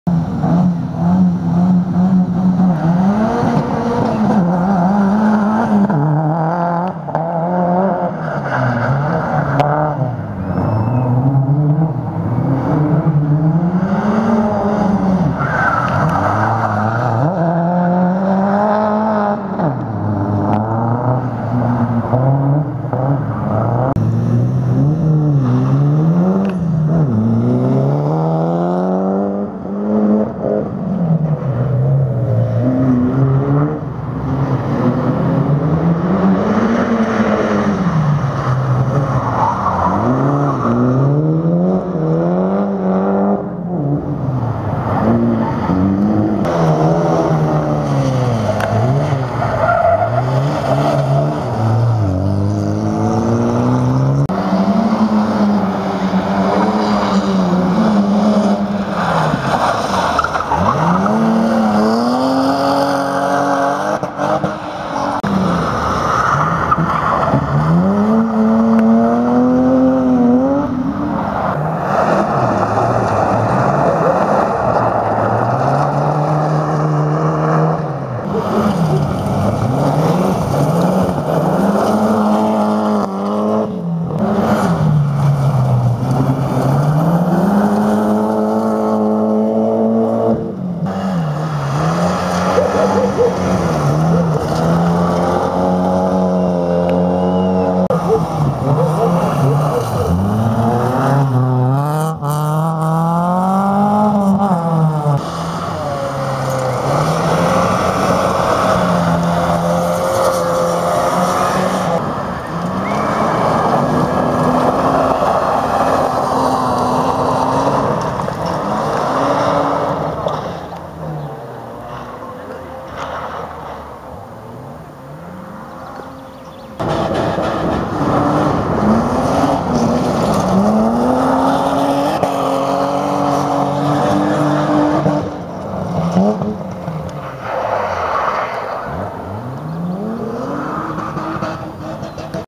►AMATEURS RALLYSPRINT | MAXIMUM ATTAC, ACTION, DRIFTING | HAAPSALU RAHVASPRINT 2014 VIDEO HD
Tulemused peale Haapsalu rahvasprinti:

Klass: 2WD-S
1. J. Jürisaar 20
2. M. Kukuškin 18
3. S. Sepp         18
4. R. Uukareda 10
5. A. Põllumäe 6

Klass: 2WD-V
1. A. Kästik 28
2. I. Tuur         12
3. G. Bogdanov 12
4. K. Tölp         10
5. V. Vorobev 8

Klass: 4WD
1. R. Parik   15
2. A. Aigro         14
3. H. Palm         13
4. R. Rannala 10
5. M. Pikkof 9

Klass: J18
1. L. Peegel         22
2. K. Kasari         20
3. K. Kaasan         15
4. M. David Tõeleid 12
5. C. Sepp                 10

Klass: ML
1. T. Kikkatalo 16
2. M. Juhe         14
3. E. Jeedas 11
4. H. Tinno         10
5. R. Saarnak 7

Klass: naised
1. S. Potisepp 28
2. A. Lepp         16
3. T. Ehrbach 14
4. M. Aarn         11
5. M. Mäekivi 10

Klass: SU
1. K. Kruusma 24
2. M. Värva 18
3. S. Ilves   16
4. U. Roos         13
5. E. Jakobi 13